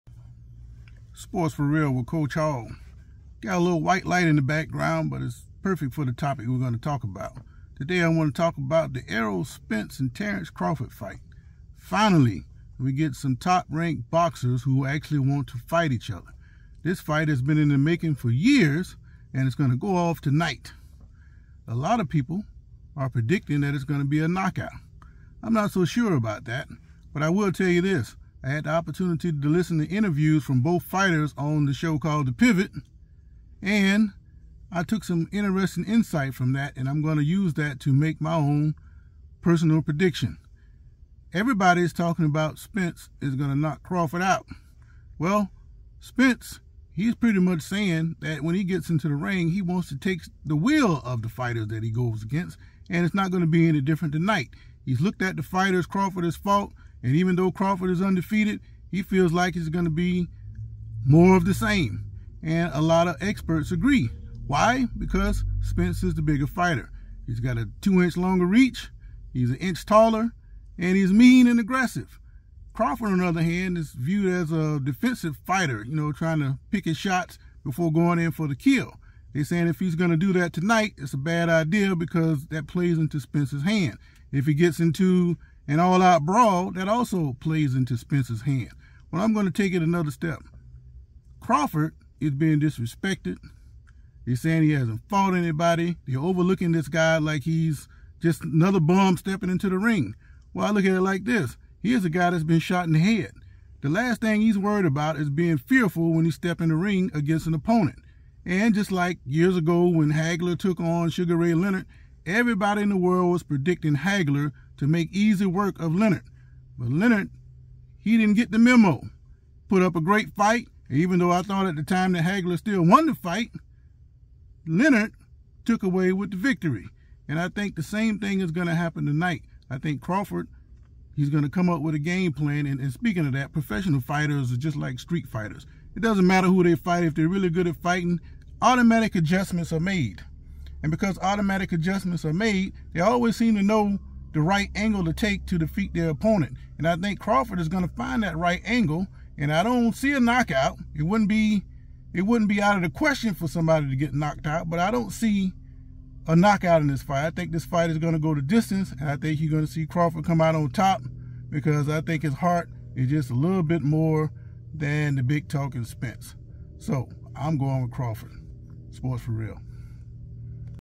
Spence vs. Crawford Fight Will Produce Thrilling Results, Just Not The One That Most Expect!
It has occurred to me over the years that the very best of professional fighters never fight someone considered an equal. That won't be the case tonight as The Crawford/Spence fight will finally take place. Most experts tend to see this fight just one way. However, I  see it completely different.